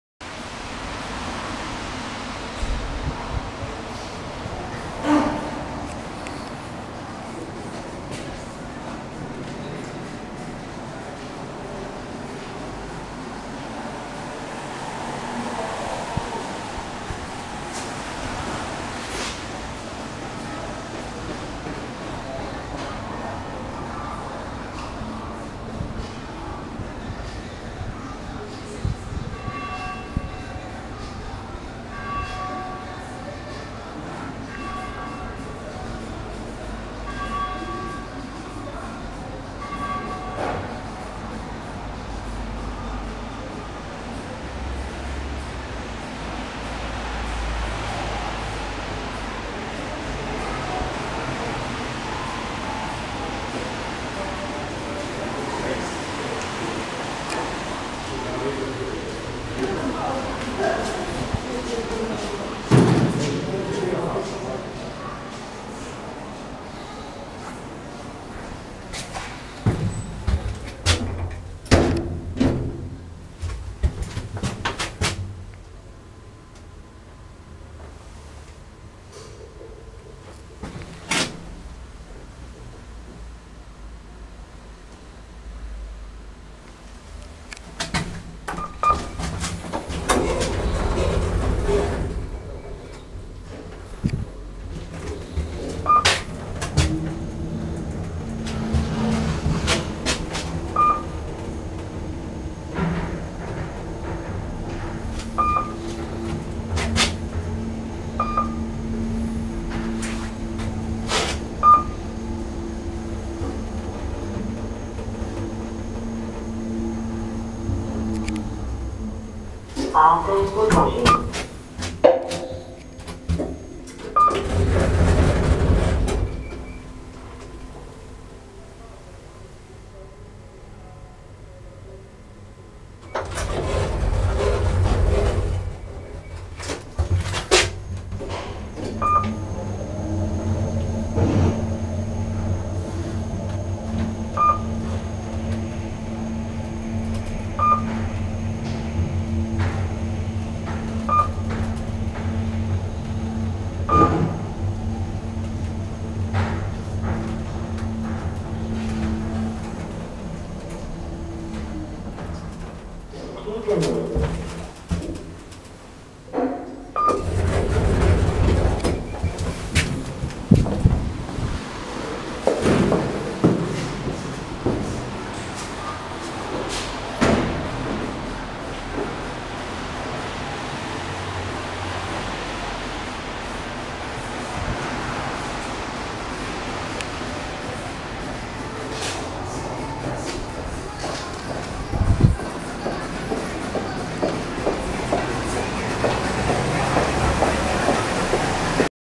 Prag, Restaurant T-Anker
Hersteller / Brand: unbekannt / unknown
Baujahr / Year of Construction: unbekannt / unknown
Modernisiert / Modernized: Kone 2013
Tragfähigkeit / Capacity: 7 Pers.
Etagen / Floors: 4 (0, 2, 4, 5)

Das Restaurant befindet sich im fünften Stock des Kaufhauses Kotva und kann durch einen Nebeneingang erreicht werden. Ob der Aufzug von Anfang an nur in die zweite, vierte und fünfte Etage gefahren ist oder erst seit der Modernisierung, habe ich nicht herausgefunden.

The restaurant is located on the fifth floor of the Kotva department store and can be reached through a side entrance. I didn´t find out if the elevator has only served the second, fourth and fifth floor from the beginning or if this has changed with the modernization.